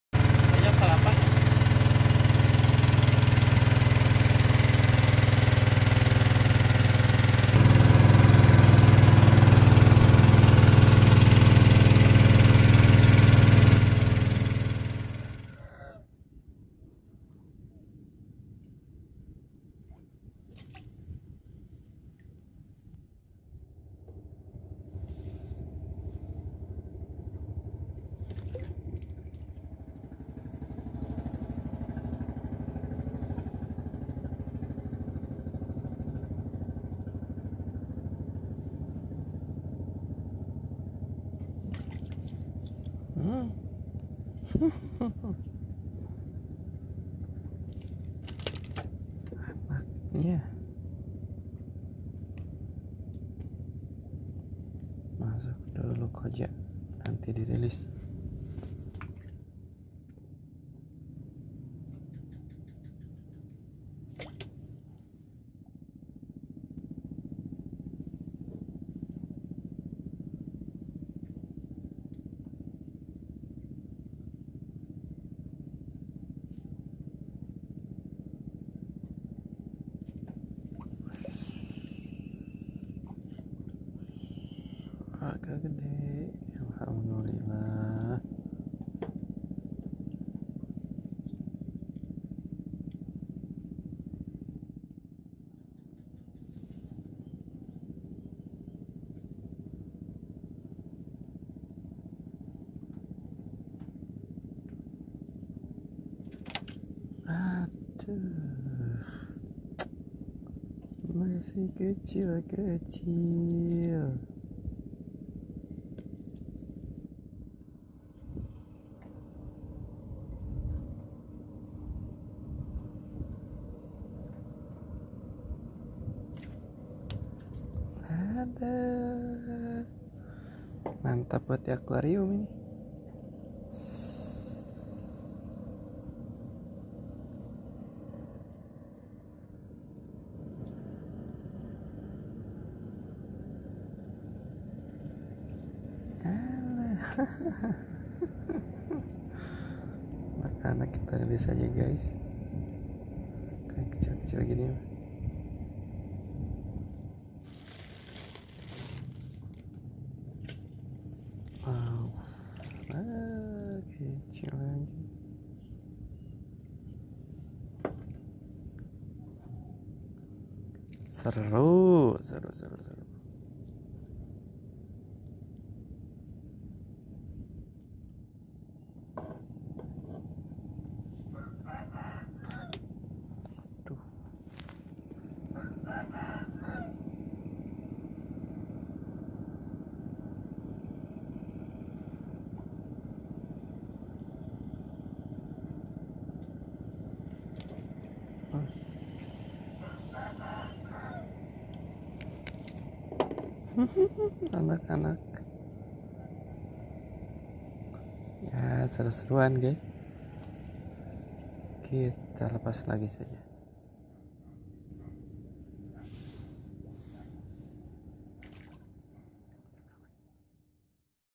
Red Devil Jadi Ikan Hias untuk di Aquarium Spot Calingcing Cirata| SHM Eps. 06
Niatnya mau mancing ikan gede, eh malah dapat ikan cantik buat di aquarium. Tadinya mau dureleas, ahh bawa aja ke rumah.